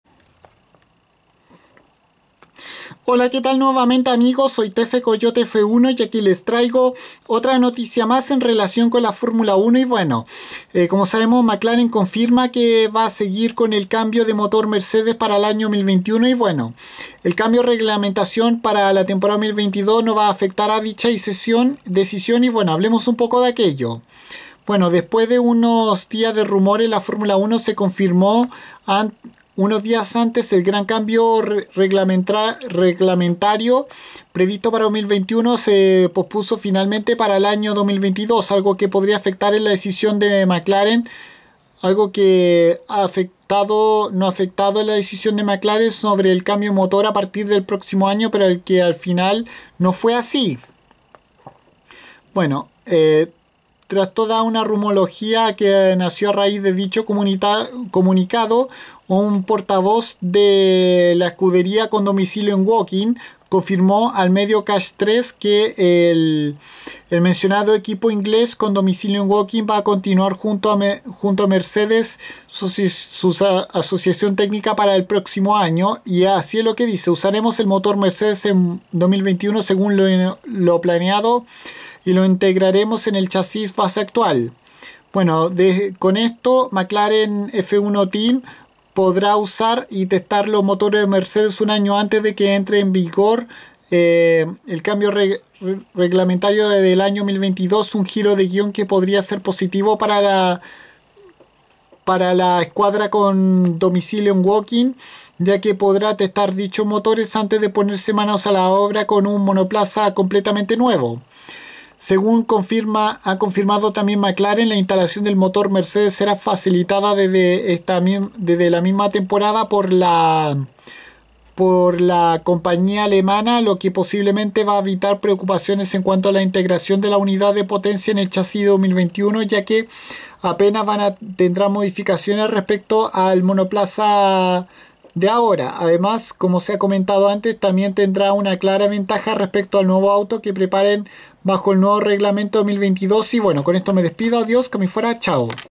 Mclaren continúa con cambiarse a motores Mercedes Benz para 2021
La escuderia inglesa con domicilio en Woking sigue en que en ese año cambiarán su proveedor de profulsores a pesar de que el cambio reglamentario se aplazó para 2022